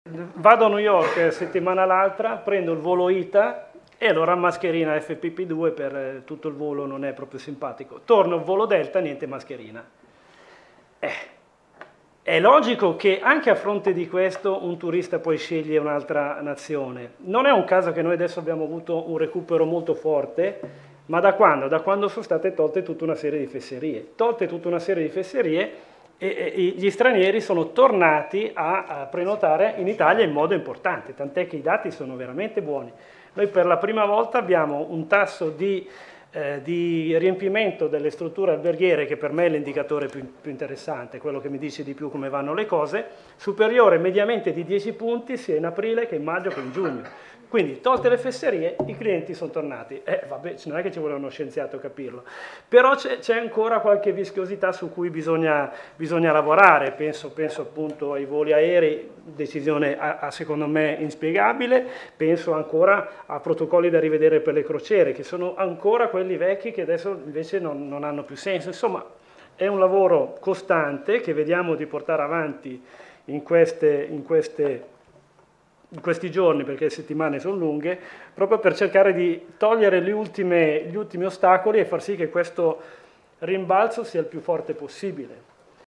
Garavaglia: "L'obbligo di mascherine sui voli italiani è inspiegabile"
(Agenzia Vista) - Roma, 18 Maggio 2022 - "Per quanto riguarda le mascherine, l'obbligo solo sui voli italiani è inspiegabile, gli stranieri stanno tornando a prenotare le vacanze in Italia, per cui è necessario rivedere i protocolli". Lo ha detto Massimo Garavaglia, ministro del Turismo, nel corso di un incontro pubblico promosso dalla Cna, la Confederazione Nazionale dell'Artigianato e della Piccola e Media Impresa.